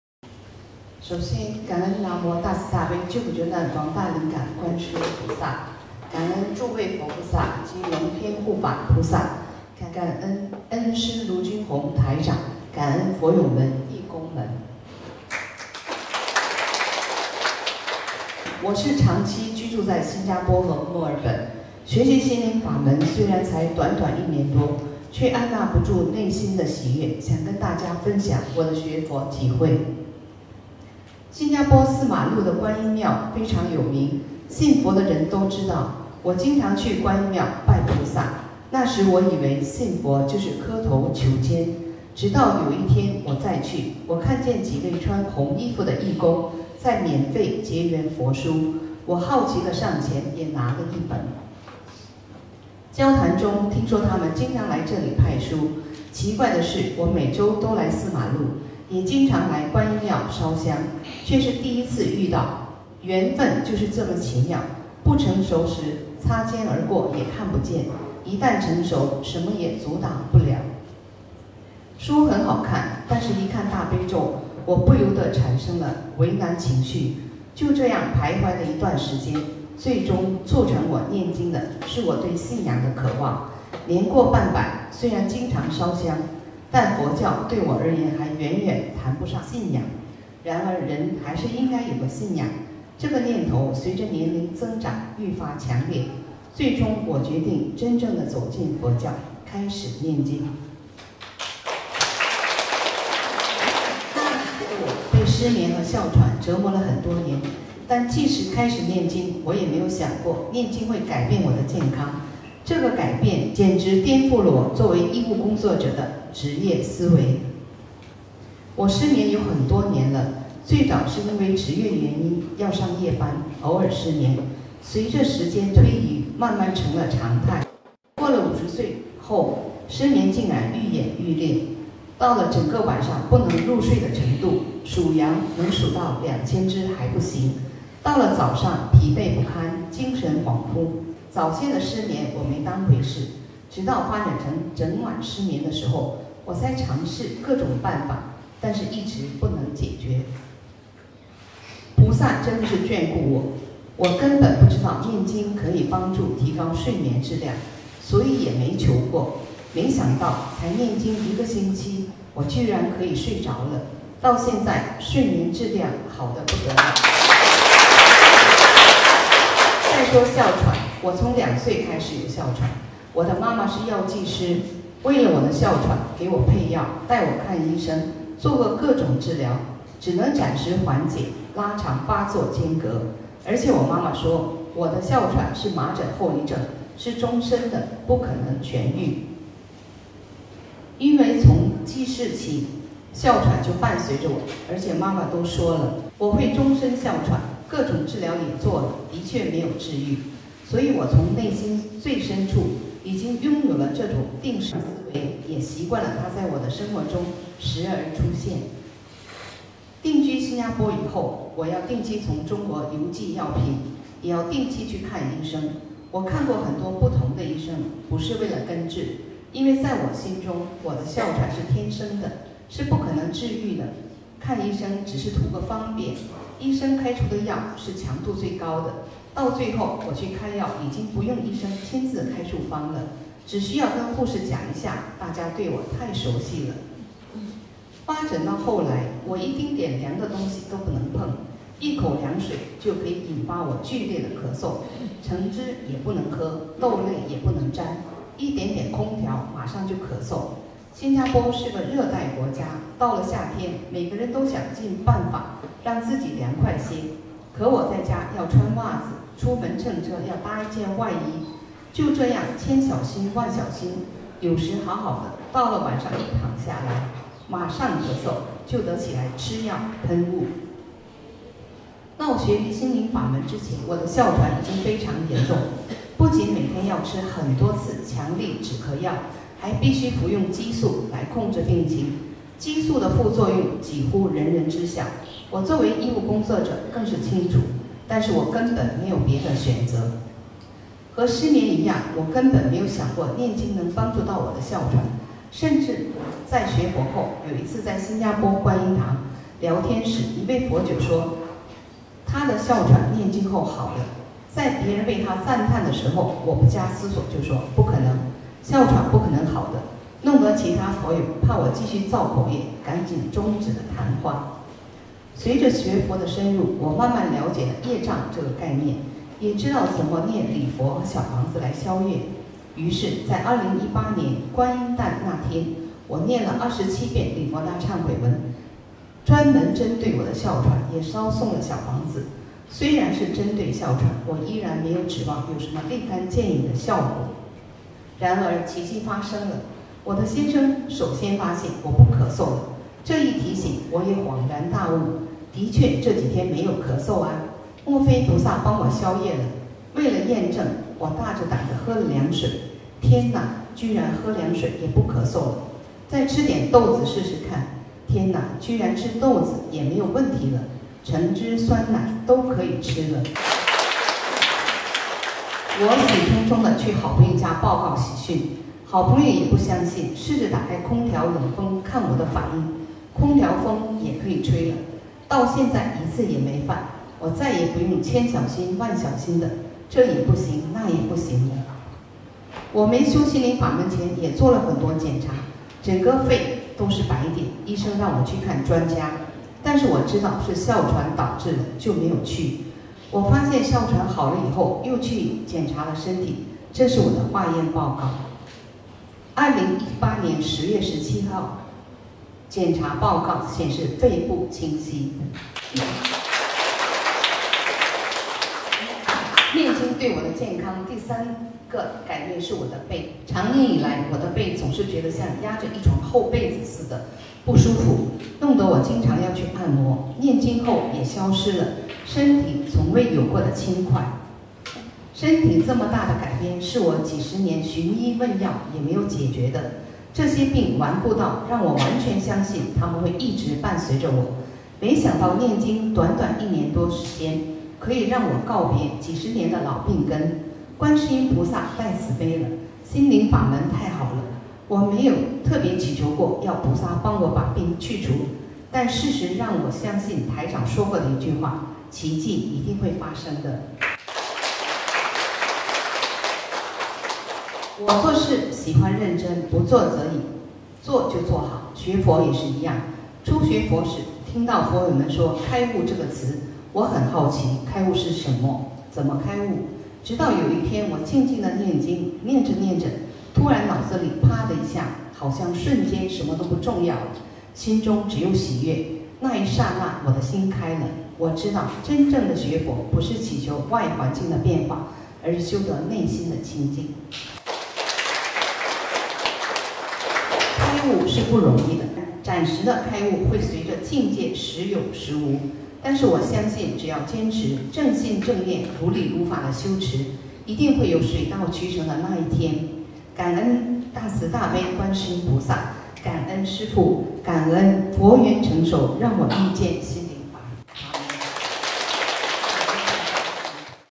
心灵法门真实不虚！学佛不到两年，五十年哮喘奇迹痊愈
【学佛改变人生】——学佛不到两年，五十年哮喘奇迹痊愈

我是新加坡的同修，现在居住在墨尔本，学习心灵法门虽然才短短一年多，却按捺不住内心的喜悦想跟大家分享我的学佛体会。

新加坡有个四马路，四马路的观音庙非常有名，信佛的人都知道，我经常去观音庙拜菩萨，那时我以为信佛就是磕头求签，直到有一天我再去，看见几个人在免费结缘佛书，我好奇的上前也拿了一本，交谈中听说她们经常来这里派书，奇怪的是我每周都来四马路，也经常来观音庙烧香，却是第一次遇到。缘分就是这么奇妙，不成熟时擦肩而过也看不见，一旦成熟，什么也阻挡不了。

书很好看，但是一看《大悲咒》我不由得产生了畏难情绪，就这样徘徊了一段时间，最终促成我念经的，是我对信仰的渴望。年过半百，虽然经常烧香，但佛教对我而言还远远谈不上信仰，然而人还是应该有个信仰，这个念头随着年龄增长愈发强烈，最终我决定真正的走进佛教，开始念经。

那时的我被失眠和哮喘折磨了很多年，但即使开始念经我也没想过念经会改变我的健康，这个改变，简直颠覆了我作为医务工作者的职业思维。

失眠很多年了，最早是因为职业原因要上夜班，偶尔失眠，随着时间推移慢慢成了常态，过了五十岁后，失眠竟然愈演愈烈，到了整晚不能入睡的程度，数羊能数到两千只还是不行，到了早上疲惫不堪，精神恍惚，早先的失眠我没当回事，直到发展成整晚失眠的时候，我才尝试各种办法，但是一直不能解决。菩萨真的是眷顾我，我根本不知道念经可以帮助提高睡眠质量，所以也没求过，没想到，才念经一个星期，我居然可以睡着了，到现在，睡眠质量好得不得了！

再说哮喘，我从2岁开始有哮喘，我的妈妈是药剂师，为了我的哮喘，给我配药，带我看医生，做过各种治疗，只能暂时缓解，拉长发作间隔。妈妈说，我的哮喘是麻疹后遗症，是终身的，不可能痊愈。因为从记事儿起，哮喘就伴随着我，而且妈妈都说了，我会终身哮喘，各种治疗也做了，的确没有治愈，所以我从内心最深处，已经拥有了这种定式思维，也习惯了它在我的生活中时而出现。定居新加坡以后，我要定期从中国邮寄药品，也要定期去看医生，我看过很多不同的医生，不是为了根治，因为在我心中，我的哮喘是天生的，是不可能治愈的，多个医生只是图个方便。医生开出的药是强度最高的，到最后，我去开药已经不用医生亲自开处方了，只需要跟护士讲一下，大家对我都太熟悉了。发展到后来，我一丁点凉的东西都不能碰，一口凉水就可以引发我剧烈的咳嗽，橙汁也不能喝，豆类也不能沾，一点点空调马上就咳嗽，新加坡是个热带国家，到了夏天，每个人都想尽办法让自己凉快些，可我在家要穿袜子，出门乘车要搭一件外衣，就这样千小心万小心，有时好好的，到了晚上一躺下来马上咳嗽，就得起来吃药喷雾。到学习心灵法门之前，我的哮喘已经非常严重，不仅每天要吃很多次强力止咳药，还必须服用激素来控制病情。激素的副作用几乎人人知晓，我作为医务工作者更是清楚，但是我根本没有别的选择。

和失眠一样，我根本没想过念经能帮助到我的哮喘，甚至在学佛后，有一次在新加坡观音堂，聊天时，一位佛友说她的哮喘念经后好了，在别人为她赞叹的时候，我不假思索的就说，不可能的，哮喘不可能好的，弄得其他佛友怕我继续造口业，赶紧终止了谈话。
随着学佛的深入，我慢慢了解了业障这个概念，也知道了怎么念礼佛和小房子来消业。于是在今年四月观音诞那天，我念了27遍礼佛大忏悔文，专门针对我的哮喘，也烧送了小房子。虽然是针对哮喘，我依然没有指望有什么立竿见影的效果，然而奇迹发生了，我的先生首先发现，我不咳嗽了！这一提醒，我也恍然大悟，的确这几天没咳嗽啊，莫非菩萨帮我消业了？为了验证，我大着胆子喝了凉水，天呐，居然喝凉水也不咳嗽了，再吃点豆子试试看，天呐，居然吃豆子也没问题了！橙汁，酸奶，都可以吃了！我喜冲冲的去好朋友家报告喜讯，好朋友也不相信，试着打开空调冷风看我的反应，空调风也可以吹了！到现在一次也没犯，我再也不用千小心万小心的这也不行那也不行了！

念经对我的健康第三个改变是我的背，常年以来，我的背总是觉得像压着一床厚被子似的不舒服，弄得我经常要去按摩，念经后也消失了，身体从未有过的轻快！
身体这么大的改变是我几十年寻医问药也没有解决的，这些病顽固到让我完全相信它们会一直伴随着我，没想到念经短短一年多时间，可以让我告别几十年的老病根，观世音菩萨太慈悲了！心灵法门太好了！我没有特别祈求过要菩萨帮我把病去除，但事实让我相信台长说过的一句话“奇迹一定会发生的！”

我做事喜欢认真，不做则已，做就做好，学佛也是一样，初学佛时听到佛友们说开悟这个词，我很好奇开悟是什么，怎么开悟？直到有一天我静静的念经，念着念着，突然脑子里啪的一下，好像瞬间什么都不重要了，心中只有喜悦，那一刹那我的心开了，我知道真正的学佛不是祈求外环境的变化，而是修得内心清净，开悟是不容易的，暂时的开悟会随着境界时有时无，但是我相信只要坚持，正信正念如理如法的修持，一定会有水到渠成的那一天。

感恩大慈大悲观世音菩萨！感恩师父！感恩佛缘成熟让我遇见心灵法门！